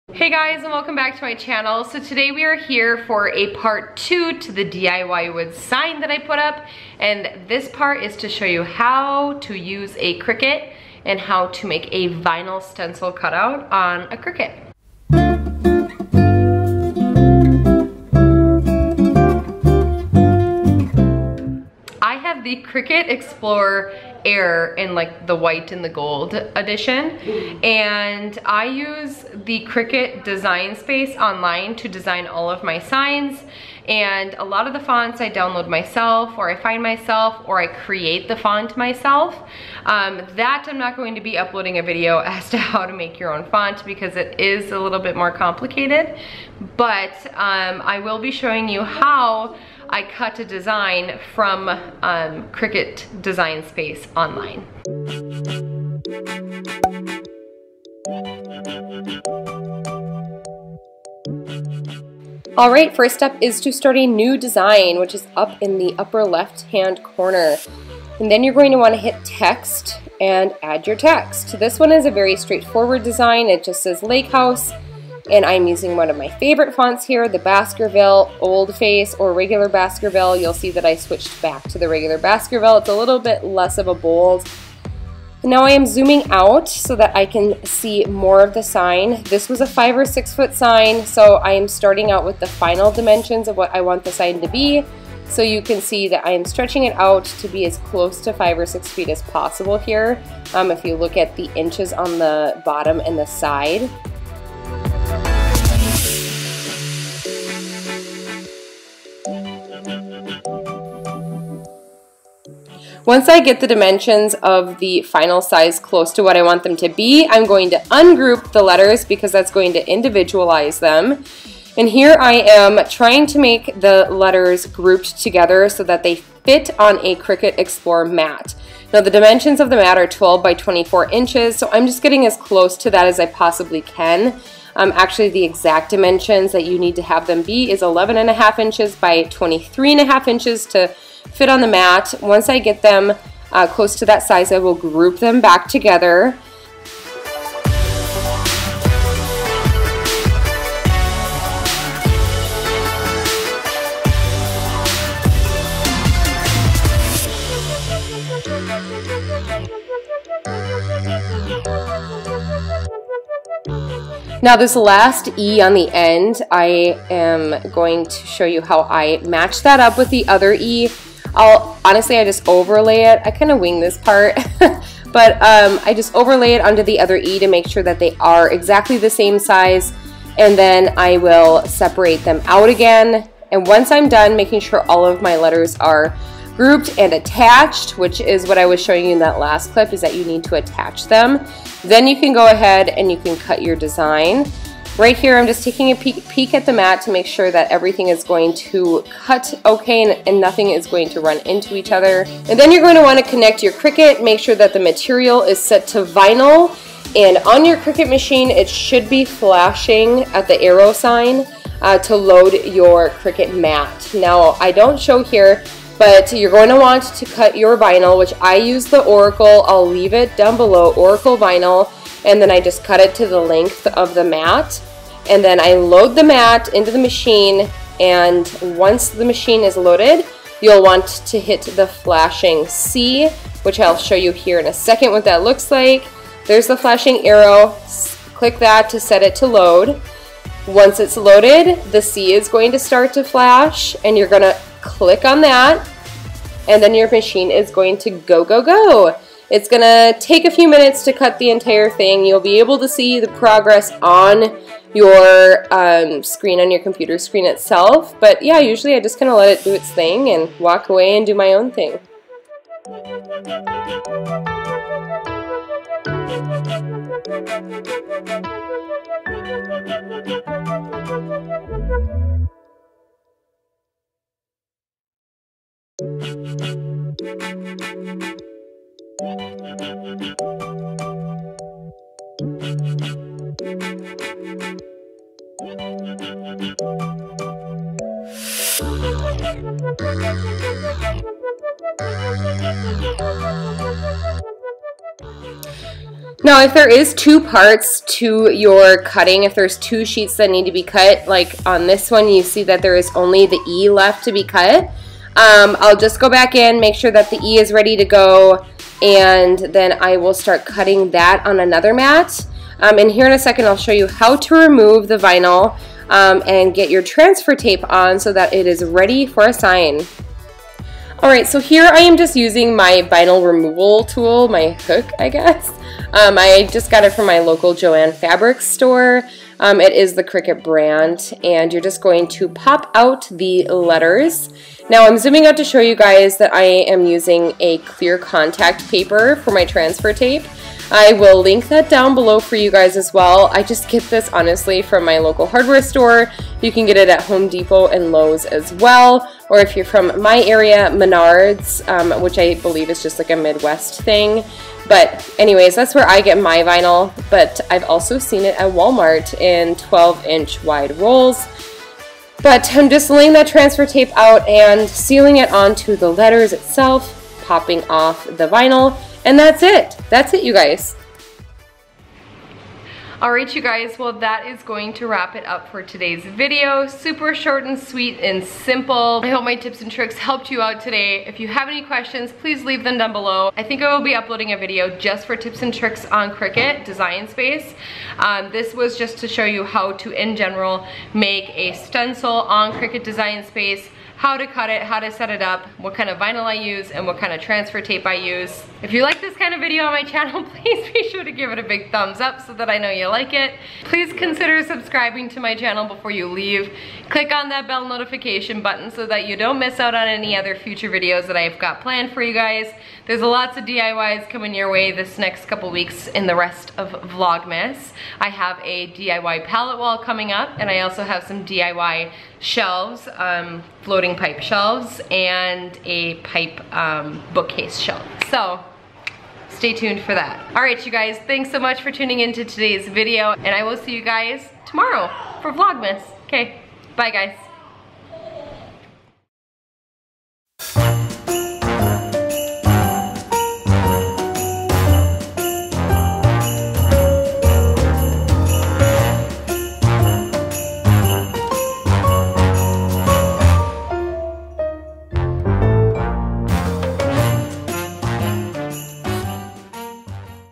DIY FARMHOUSE STYLE SIGN | HOW TO MAKE A VINYL STENCIL | CRICUT EXPLORE AIR
It's finally here!! Part 2 to how to make your own farmhouse style sign!! In this video I show you how to use a Cricut to cut out a vinyl stencil for wood signs! Below is the link for the first part of this video which starts after cutting the vinyl.

Vinyl I use:

Transfer Tape I Use:

Cricut I Have:

Vinyl Tools:

Camera I use: